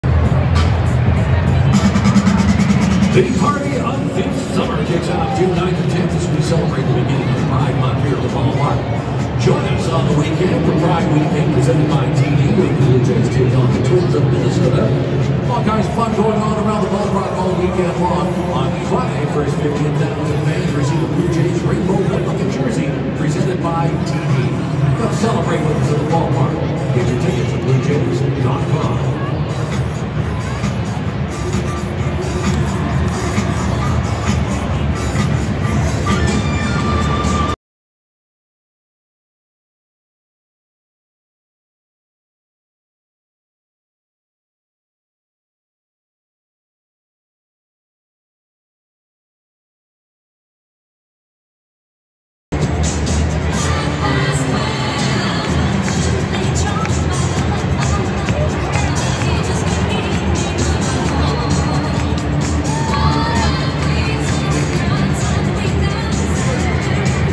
Replacing 3rd Base At Toronto Blue Jays Game Rogers Centre
The Clean Up Crew Replace 3rd & Cleans Up The Entire Field Too